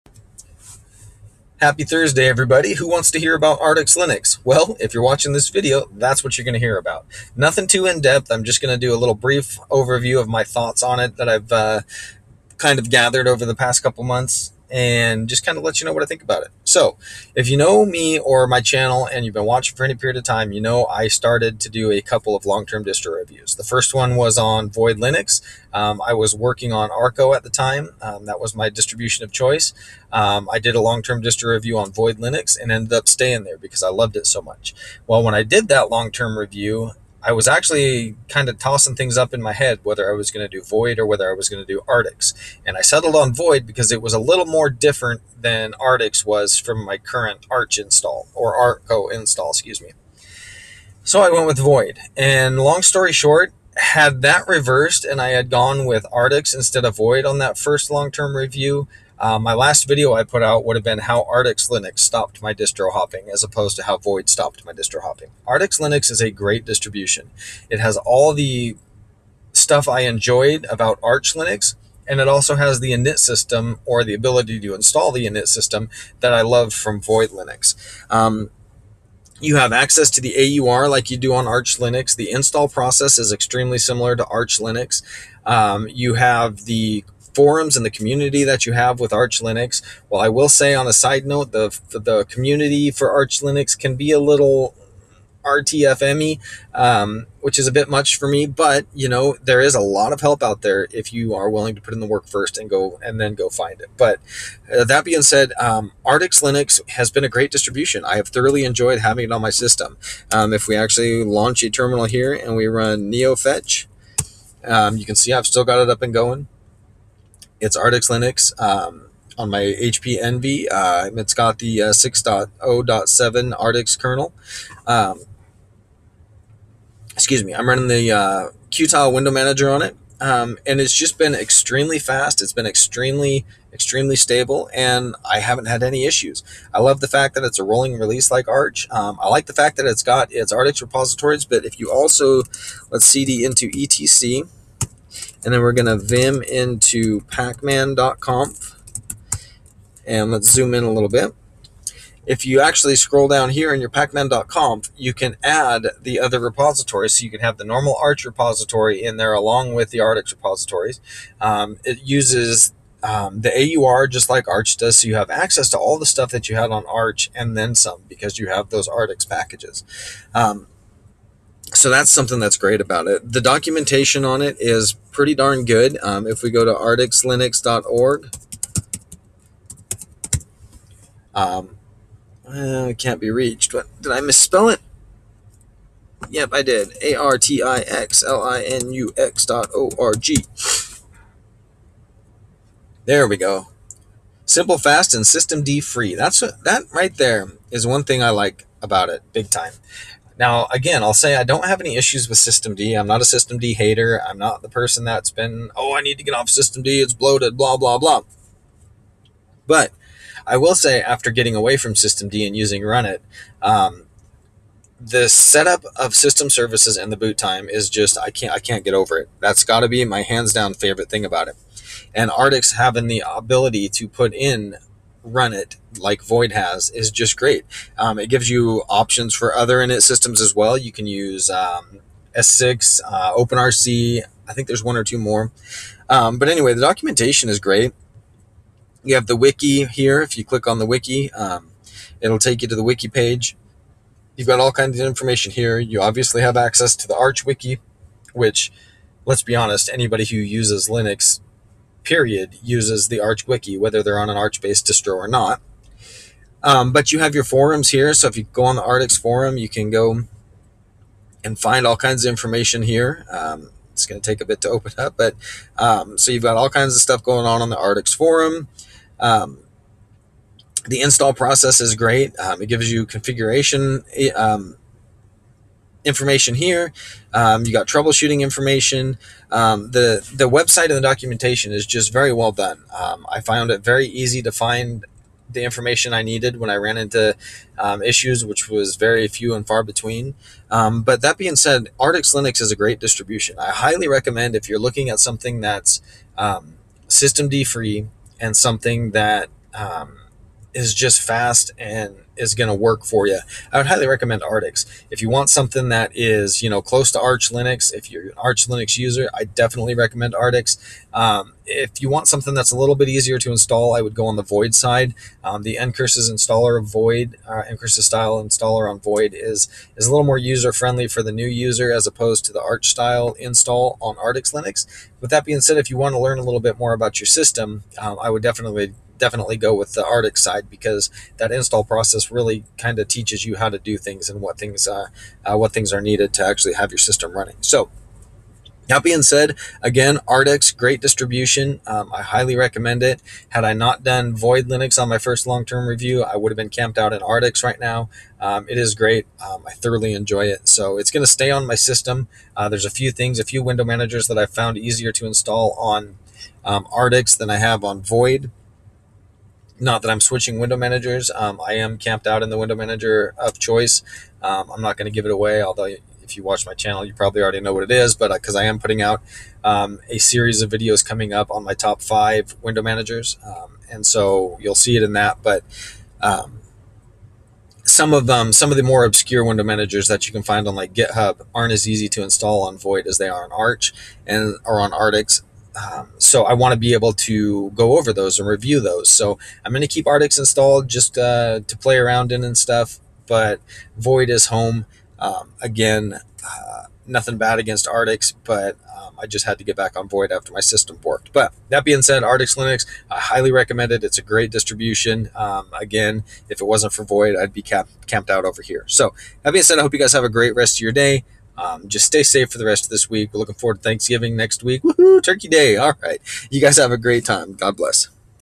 My brief thoughts on Artix linux - As good as Void, another cure for distrohopping
Gitlab